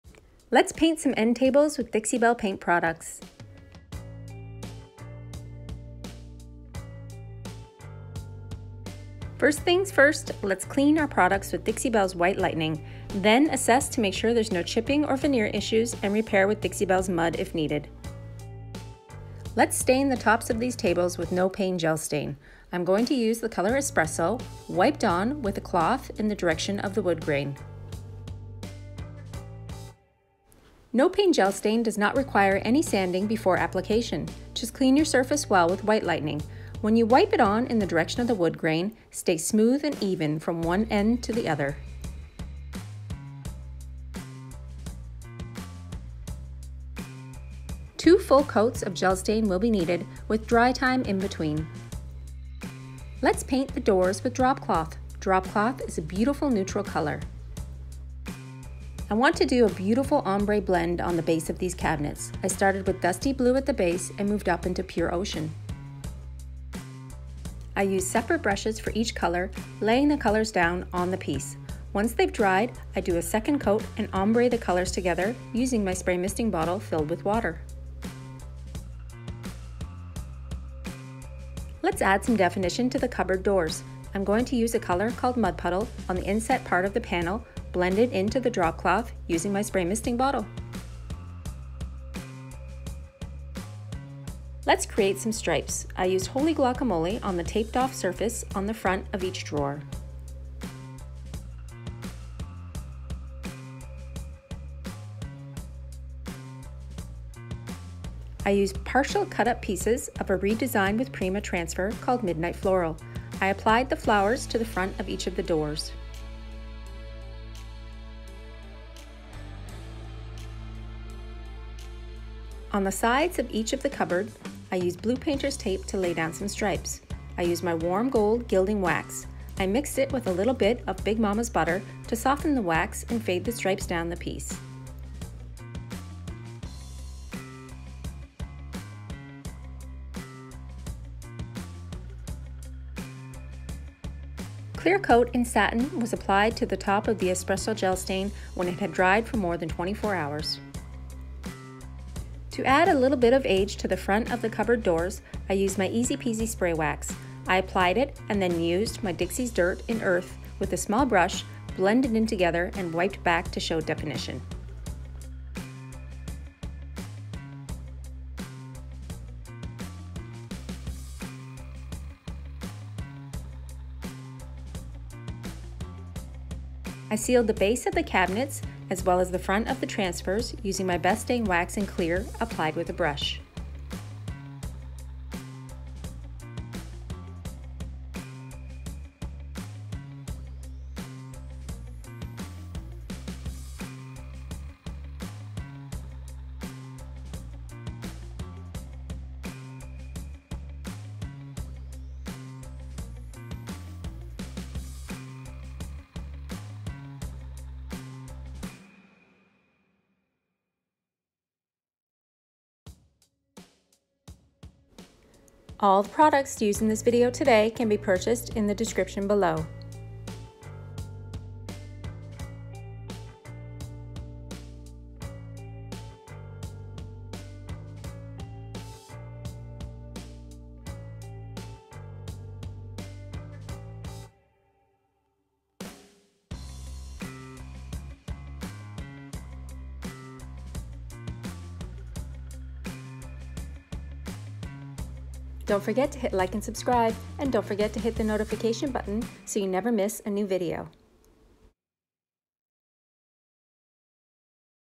How To Use Paint & Gel Stain On Wood Furniture
This is a simple how to video. Step by step process of painting and using gel stain. This makeover was completed using all Dixie Belle paint products. Follow along while we makeover some wood furniture!

Colors used:
Espresso Gel Stain
Clear Coat
Best Dang Wax
ReDesign With Prima Transfer
Dusty Blue
Pure Ocean
Holy Guacamole
Gold gilding wax
Drop Cloth
Mud Puddle

These products can be purchased here!